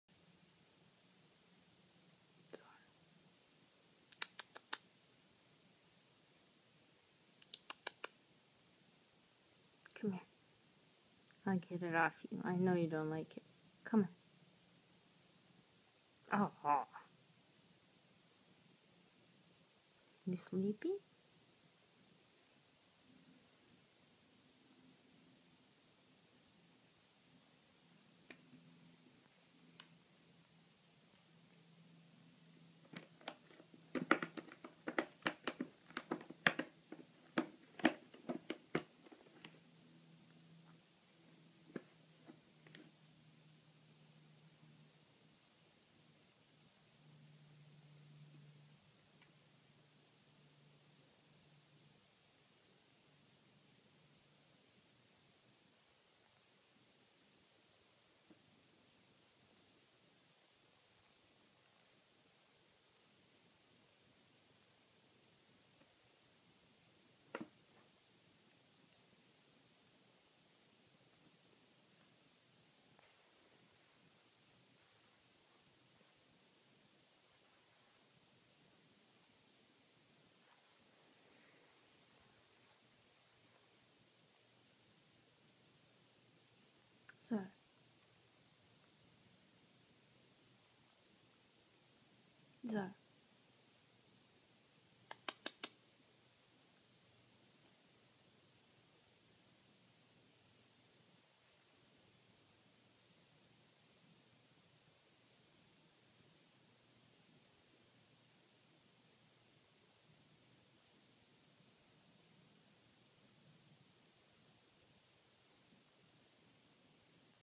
Snowshoe cat in harness
My poor little kitty did not understand what was going on when I put the harness on her!! Buy I really want to take her out on walks in our backyard.. Have you taken your cat on walks?